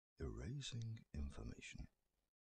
Erasing Information (1)bob
Sound Bytes, for games, missions, to video and audio editors. These are some custom soundbytes to play with.
Please credit  Wraith and Friends Productions if you use these.